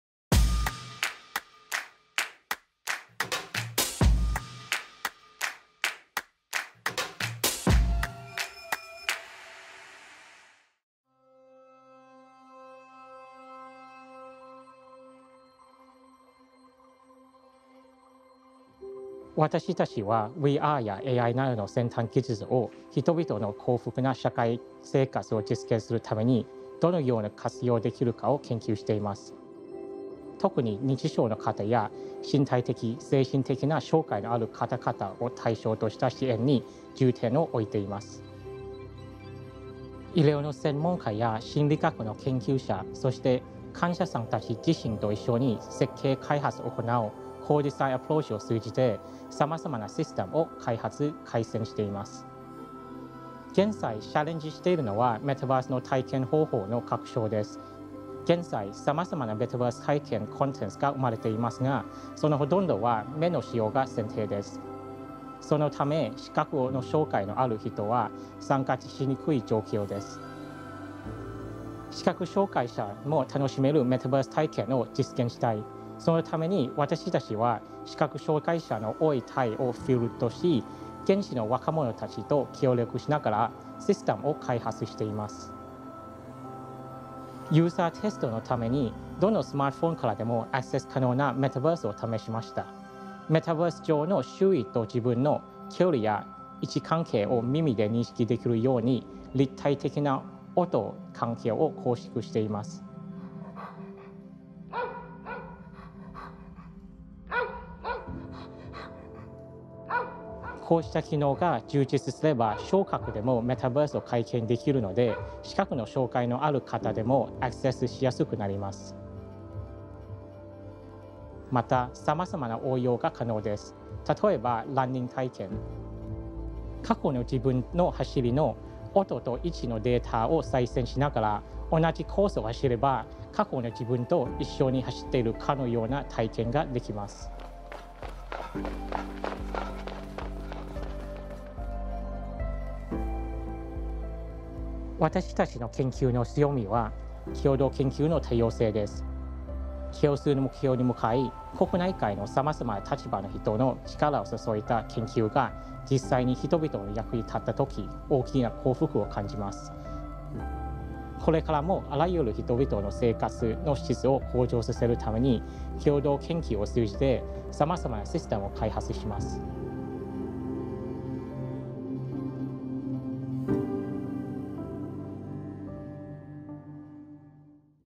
Research at Kyoto Institute of Technology Vol.27 -- Assoc. Prof. SIRIARAYA Panote
"Extrasensory Experience: A Metaverse for the Visually Impaired"